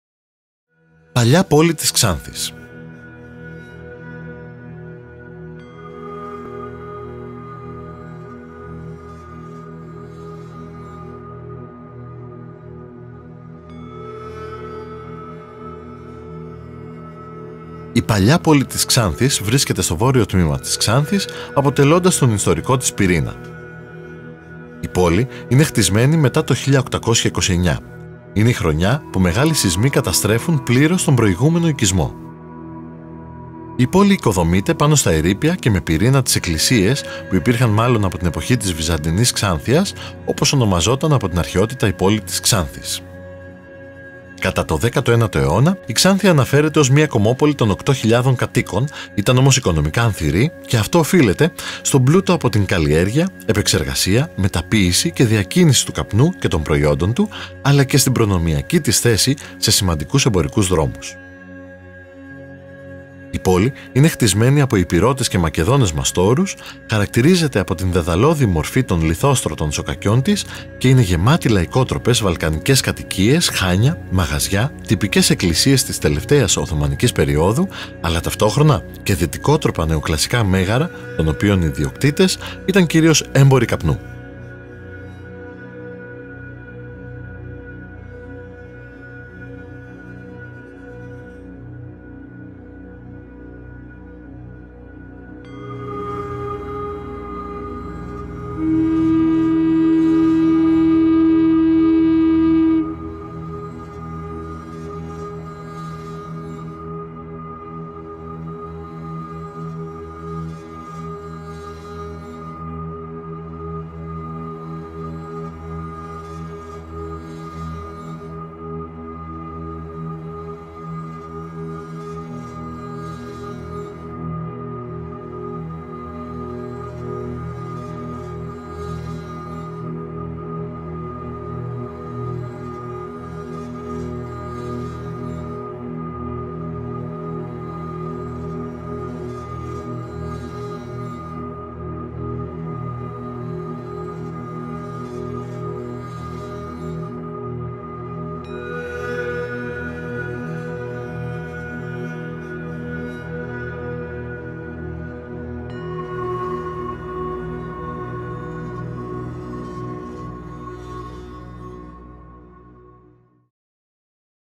Παλιά Πόλη της Ξάνθης
Η Παλιά Πόλη της Ξάνθης βρίσκεται στο βόρειο τμήμα της Ξάνθης αποτελώντας τον ιστορικό της πυρήνα.

Η πόλη είναι χτισμένη μετά το 1829. Είναι η χρονιά που μεγάλοι σεισμοί καταστρέφουν πλήρως τον προηγούμενο οικισμό.

Η πόλη οικοδομείται πάνω στα ερείπια και με πυρήνα τις εκκλησίες, που υπήρχαν μάλλον από την εποχή της βυζαντινής Ξάνθειας, όπως ονομαζόταν από την αρχαιότητα η πόλη της Ξάνθης.

Κατά τον 19ο αιώνα, η Ξάνθη αναφέρεται ως μια κωμόπολη των 8.000 κατοίκων, ήταν όμως οικονομικά ανθηρή και αυτό οφείλεται στον πλούτο από την καλλιέργεια, επεξεργασία, μεταποίηση και διακίνηση του καπνού και των προϊόντων του, αλλά και στην προνομιακή της θέση σε σημαντικούς εμπορικούς δρόμους.

Η πόλη είναι χτισμένη από Ηπειρώτες και Μακεδόνες μαστόρους, χαρακτηρίζεται από την δαιδαλώδη μορφή των λιθόστρωτων σοκακιών της και είναι γεμάτη λαϊκότροπες βαλκανικές κατοικίες, χάνια, μαγαζιά, τυπικές εκκλησίες της τελευταίας Οθωμανικής περιόδου αλλά ταυτόχρονα και δυτικότροπα νεοκλασικά μέγαρα των οποίων οι ιδιοκτήτες ήταν κυρίως έμποροι καπνού.